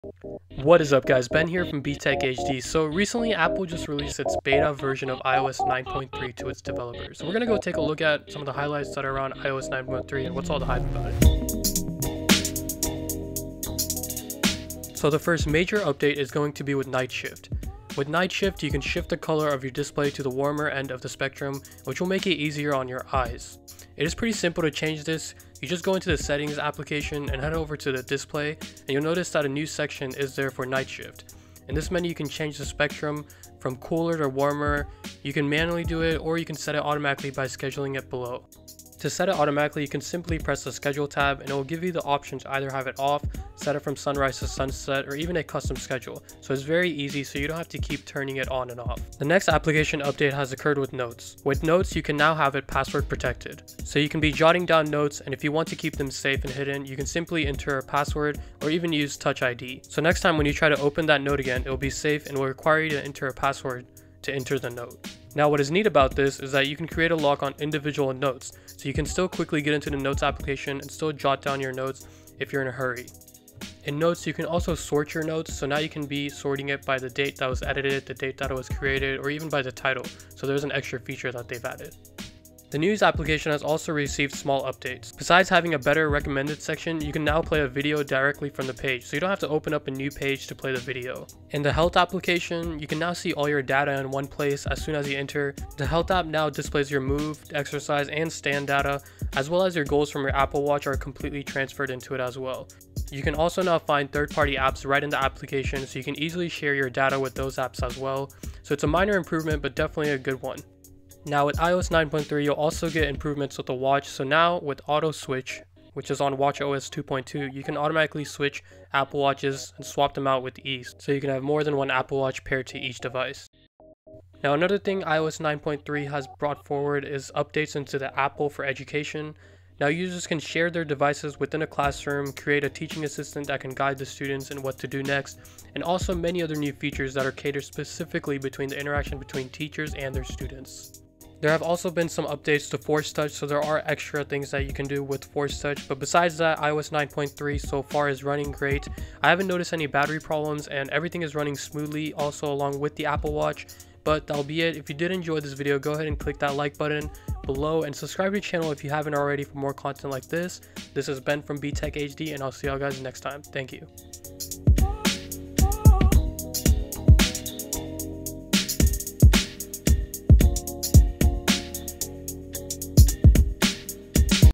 iOS 9.3 Beta New Features!
We take a look at the all new iOS 9.3 with ALL NEW of Awesome Features. Night Shift (f.Lux), Notes Passcode Lock, New 3D Touch Toggles & More!